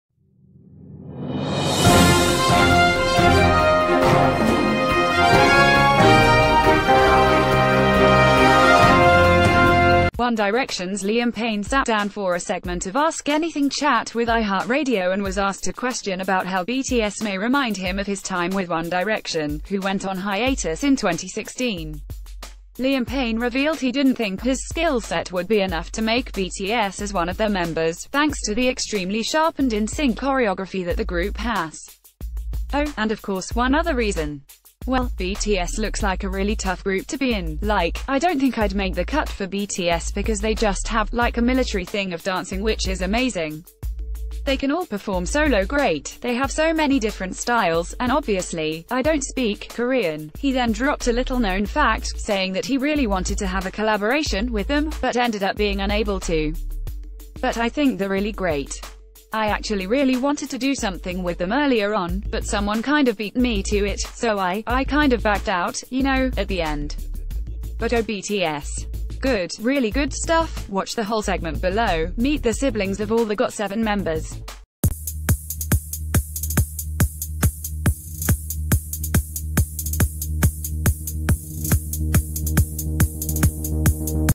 One Direction’s Liam Payne Talks About BTS’s Skill, and The Collab That Never Happened
One Direction‘s Liam Payne sat down for a segment of Ask Anything Chat with iHeartRadio and was aske...